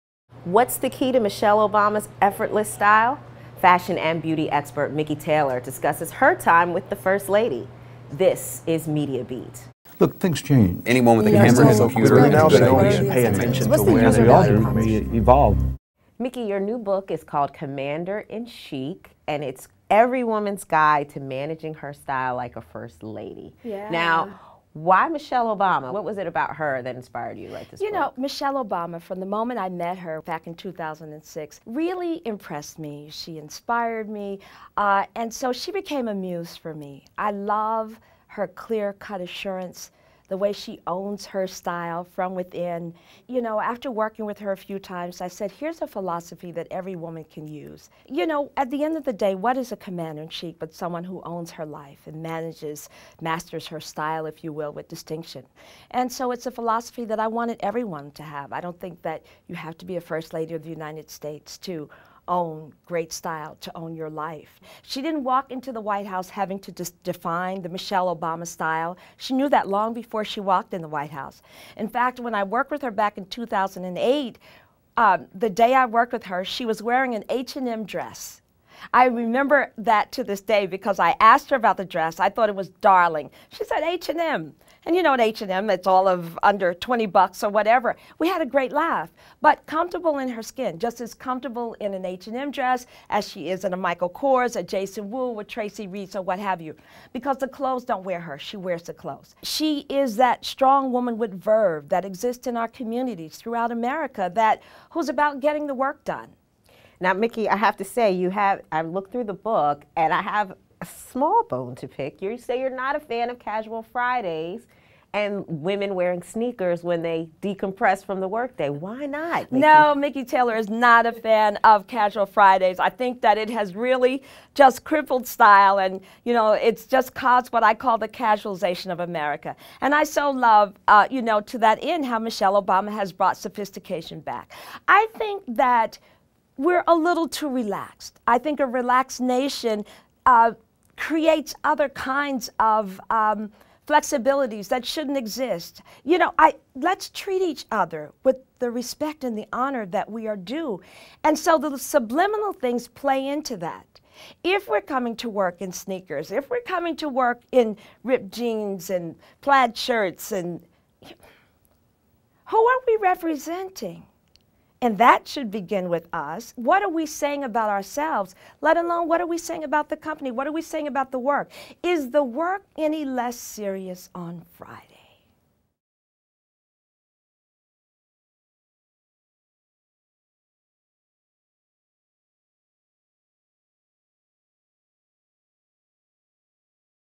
Mikki Taylor Takes on Casual Fridays - Media Beat (2 of 3)
In her new book Commander in Chic, Essence editor-at-large Mikki Taylor doles out fashion and beauty tips for the everyday woman based on examples from Michelle Obama.

"I love her clear cut assurance, the way she owns her style from within," she explained in our Media Beat interview.

And one thing FLOTUS has done, according to Taylor, is inject a much needed sophistication into America's dress code. Casual Fridays? No, thank you, she says.

"I think that we're a little too relaxed. I think a relaxed nation creates other kinds of flexibilities that shouldn't exist. Let's treat each other with the respect and the honor that we are due, and so the subliminal things play into that. If we're coming to work in sneakers, if we're coming to work in ripped jeans and plaid shirts, who are we representing?"